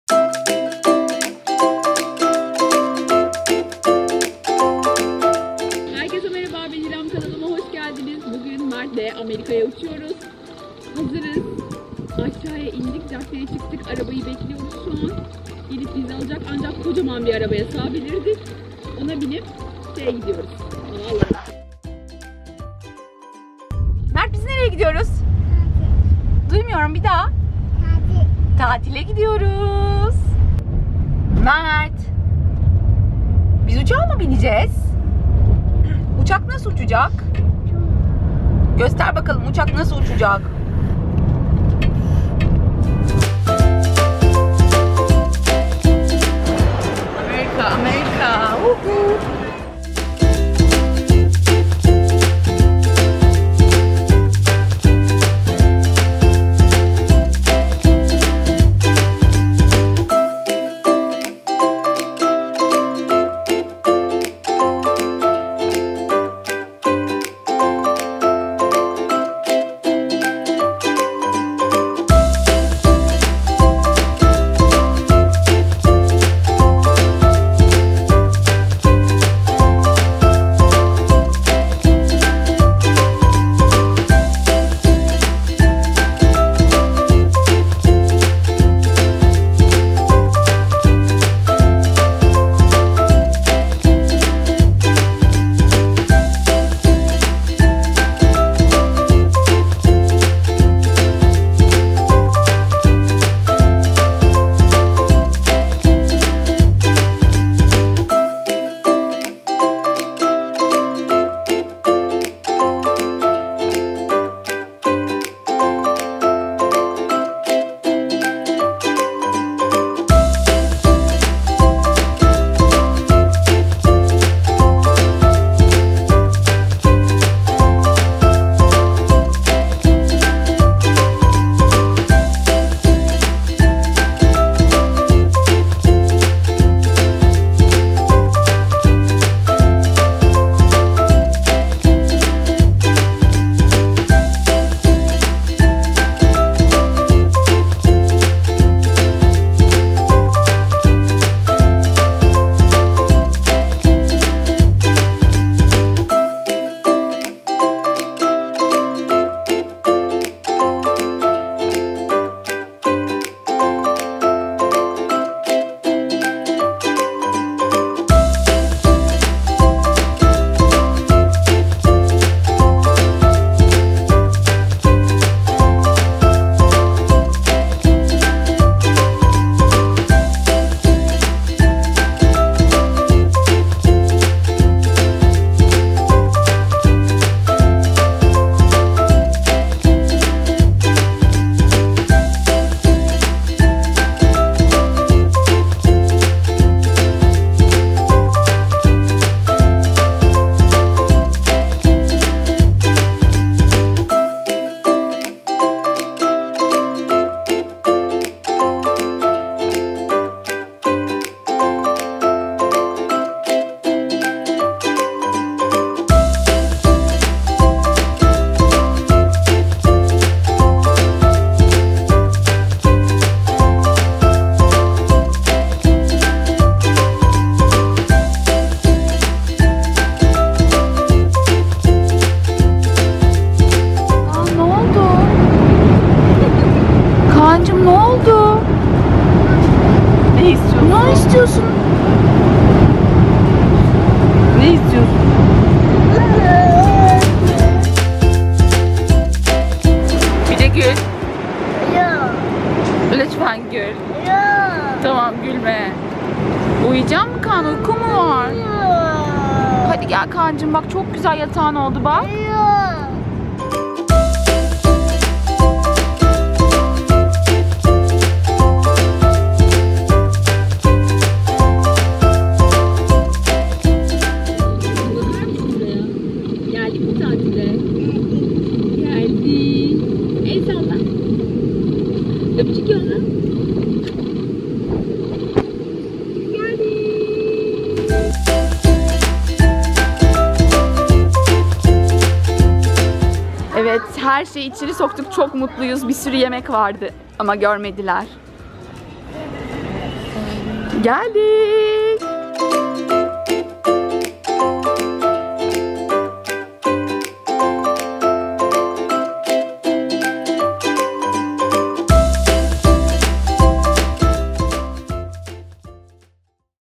Çocukla Amerika, Uçak | İrem Güzey
Bu videomda İstanbul - Miami uçuşunda başımızdan geçenleri sizlerle paylaştım. Bu uzun yolculuk dört çocuk ile sandığımızdan kolay geçti. Ya gerçekten o kadar da zor değil ya da şanslı günümüzdeydik :) Umarım videom hoşunuza gider :)

Diğer videolarımı aşağıdaki linklere tıklayıp izleyebilirsiniz.